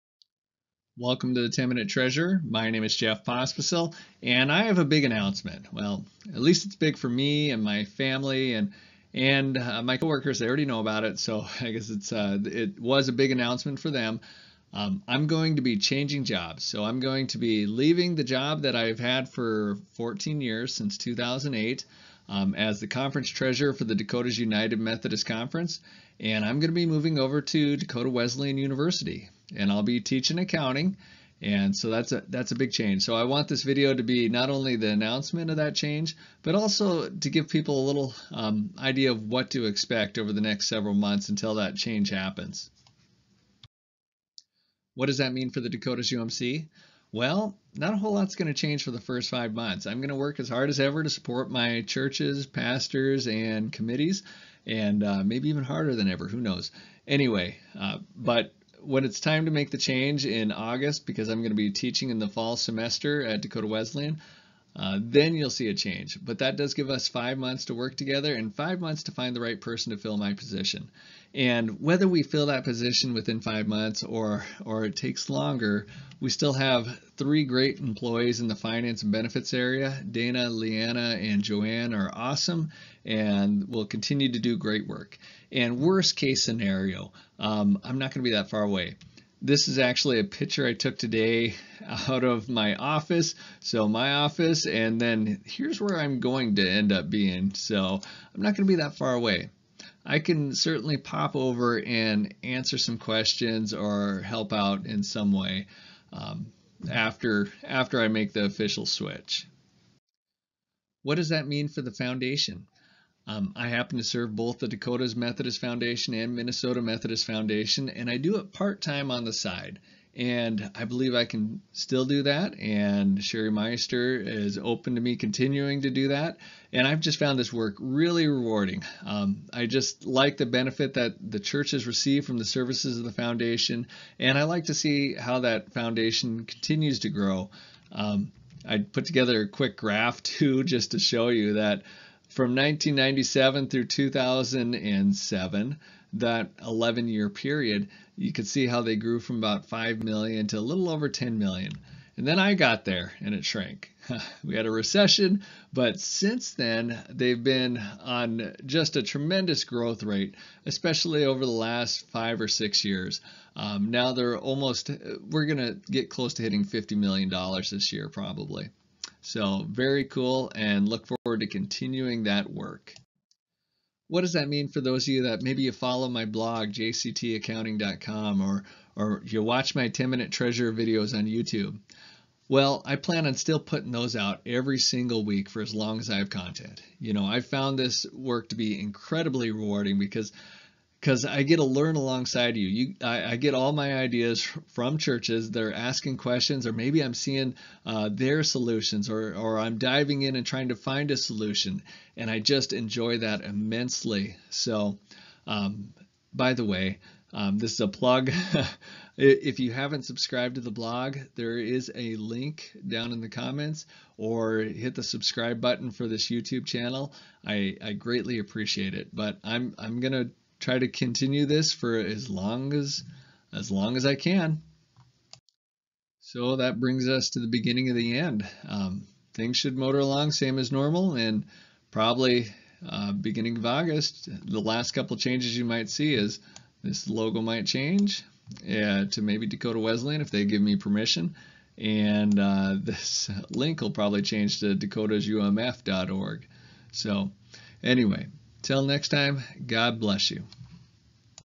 Big Announcement: I'm changing jobs!
Beginning August 15, 2022, I'll be teaching full-time at Dakota Wesleyan University!